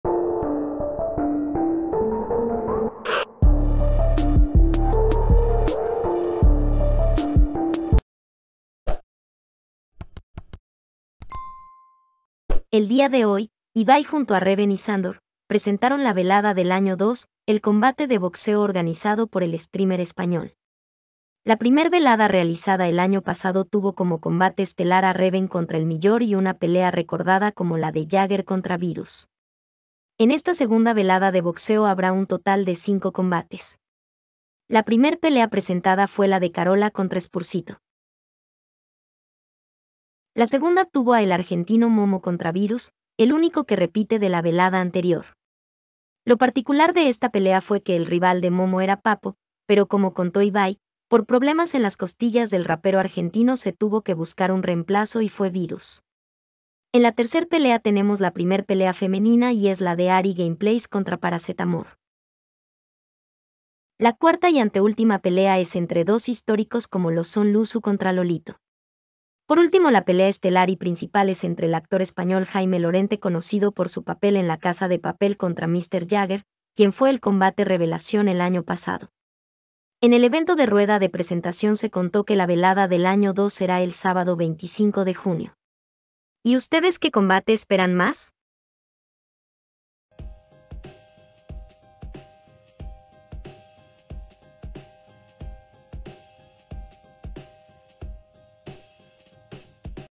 IBAI PRESENTÓ LA VELADA DEL AÑO 2 DE BOXEO CON EL COMBATE PRINCIPAL DE JAIME LORENTE VS MR JAGGER
El día de hoy Ibai junto a Reven y Sandor, presentaron la Velada del año 2,el combate de boxeo organizado por el estrímer español. La primer velada realizada el año pasado tuvo como combate estelar a Reven contra Elmillor y una pelea recordada como la de Jagger contra Viruzz. En esta segunda velada de boxeo habrá un total de 5 combates. La primer pelea presentada fue la de Carola contra Spursito. La segunda tuvo a el argentino Momo contra Viruzz, el único que repite de la velada anterior. Lo particular de esta pelea fue que el rival de momo era Papo, pero como contó Ibai, por problemas en las costillas del rapero argentino se tuvo que buscar un reemplazo y fue Viruzz. En la tercer pelea tenemos la primer pelea femenina y es la de Ari Gameplays contra Paracetamor.
La cuarta y anteúltima pelea es entre dos históricos como lo son Luzu contra Lolito. Por último la pelea estelar y principal es entre el actor español Jaime Lorente conocido por su papel en la casa de papel contra MR Jagger, quien fue el combate revelación el año pasado. En el evento de rueda de presentación se contó que la velada del año 2 será el Sábado 25 de Junio. Y ustedes que combate esperan mas?